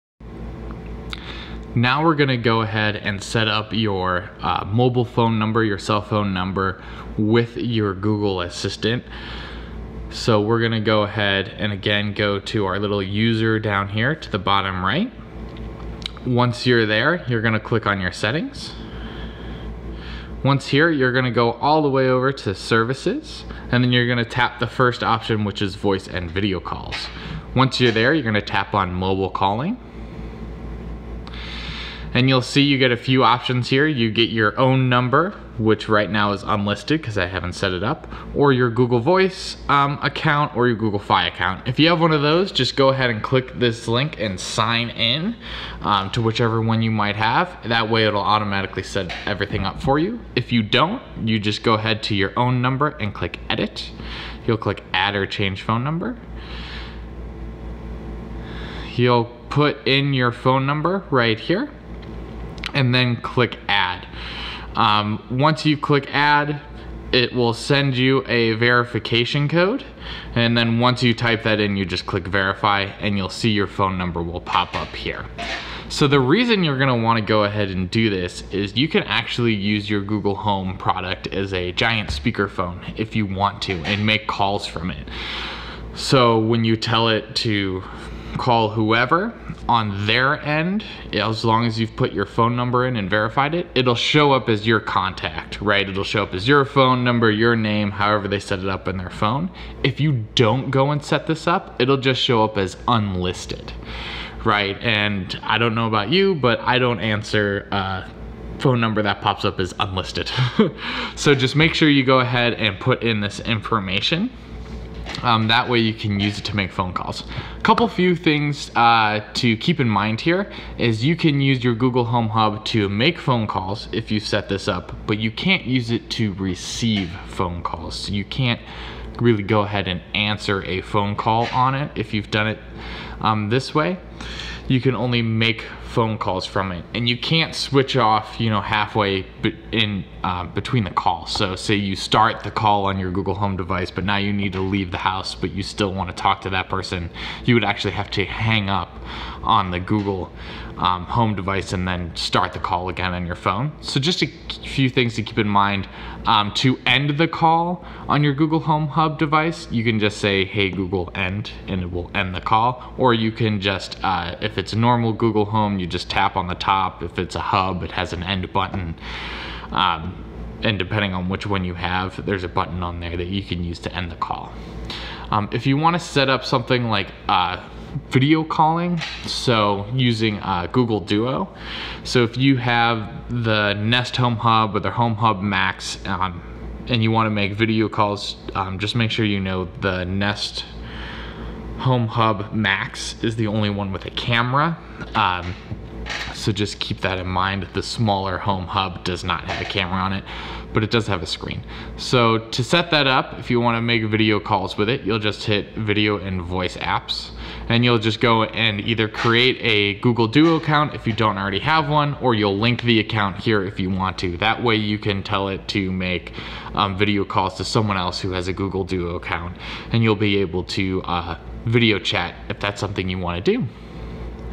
Home app: Add phone number and Duo account
How to setup your phone number so you can make calls with Google Home and how to set up your Duo account so you can make video calls.